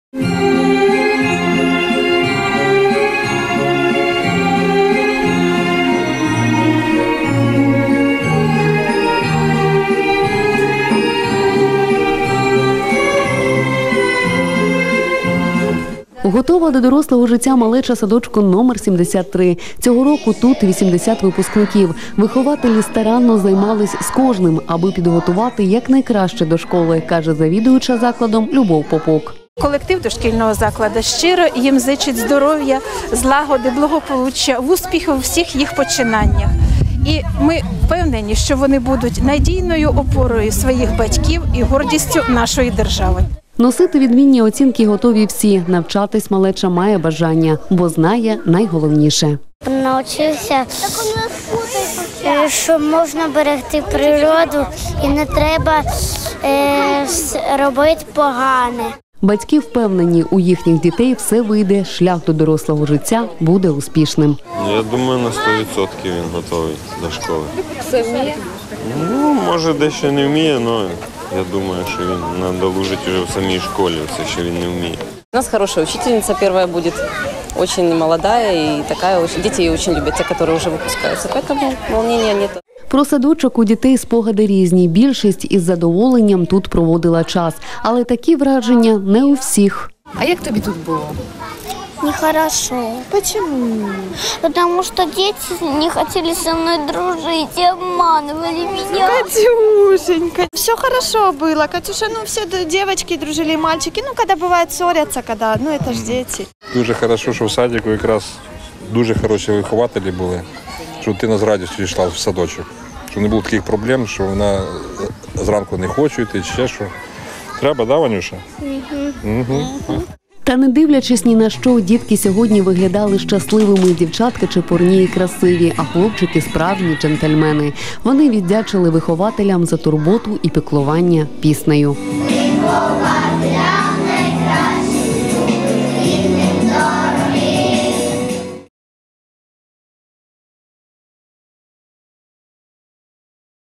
Випускні для наймолодших | Телеканал Новий Чернігів
Малюки прощаються з дитячими садками.